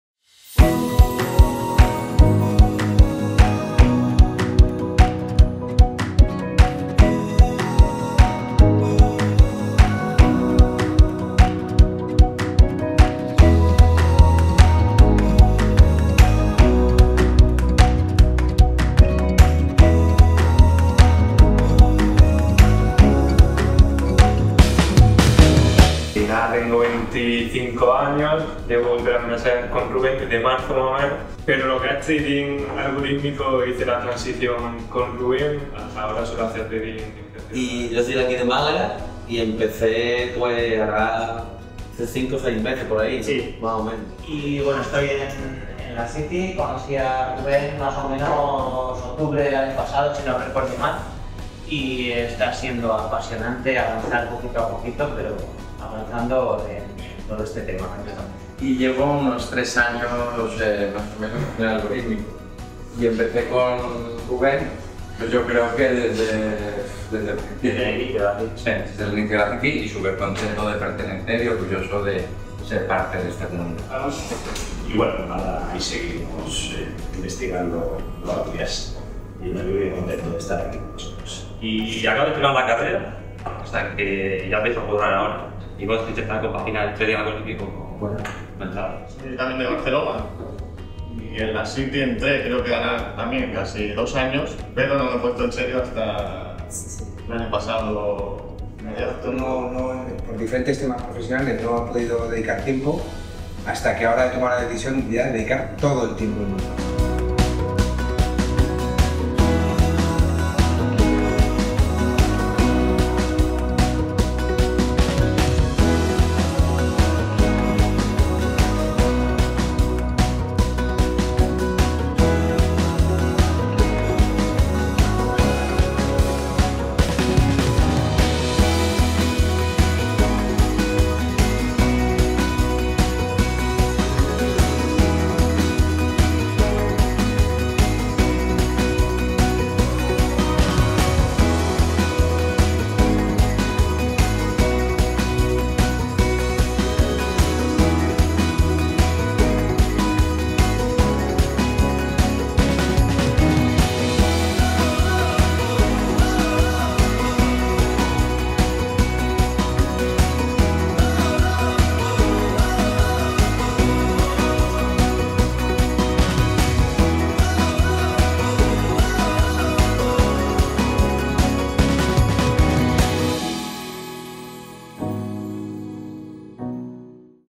Encuentro Comunidad La City - Trading Algorítmico
Un resumen de cómo vivimos el primer encuentro presencial de La City. Un día para aprender sobre trading, compartir y pasarlo bien.